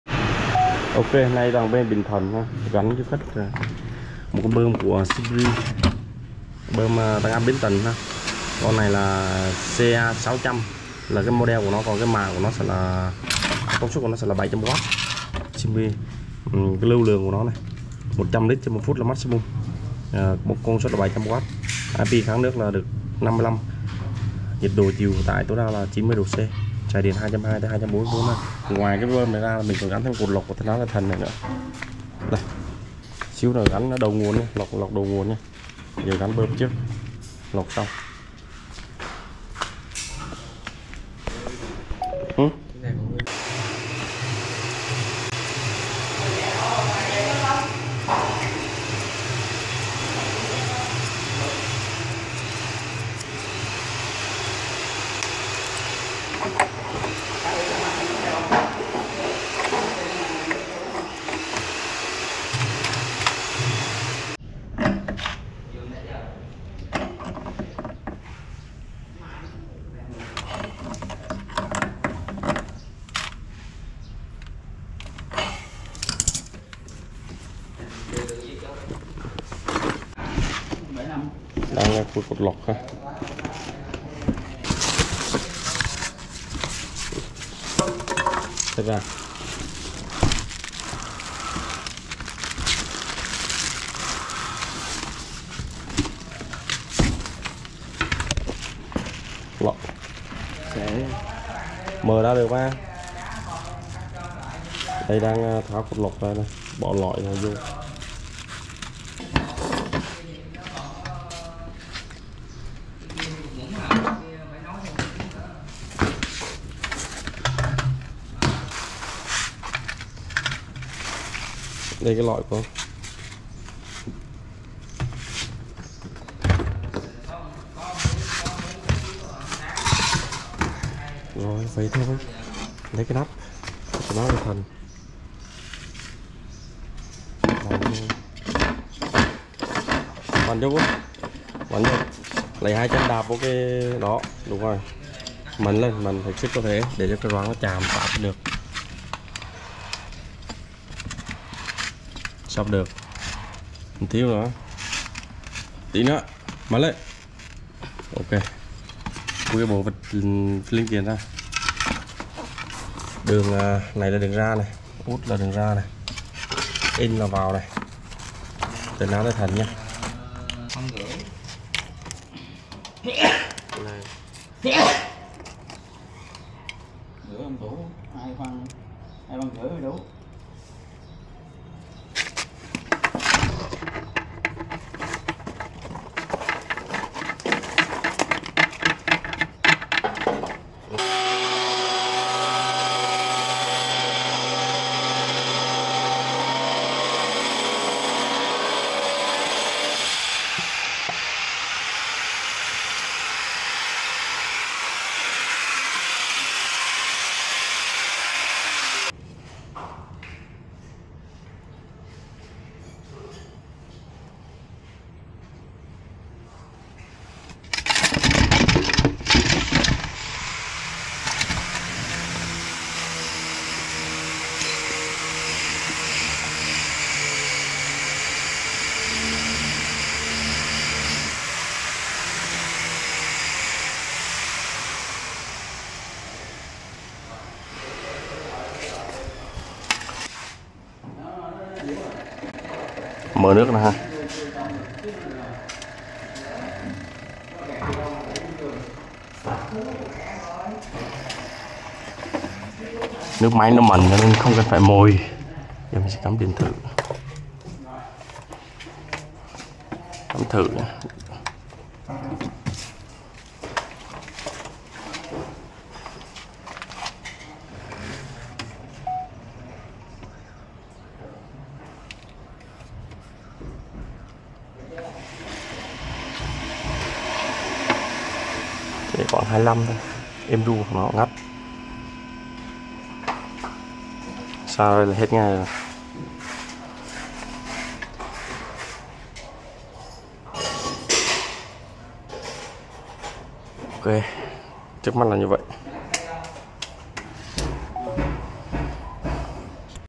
Lắp đặt máy bơm tăng áp biến tần Shimge 700W và cột lọc nước đầu nguồn Tân Á Đại Thành
Máy Bơm Tăng Áp Biến Tần Shimge 700W và Cột Lọc Nước Đầu Nguồn Tân Á Đại Thành là sự kết hợp hoàn hảo để đảm bảo nguồn nước sạch và áp lực nước mạnh mẽ cho gia đình bạn. Dưới đây là hướng dẫn chi tiết cách lắp đặt hai thiết bị này.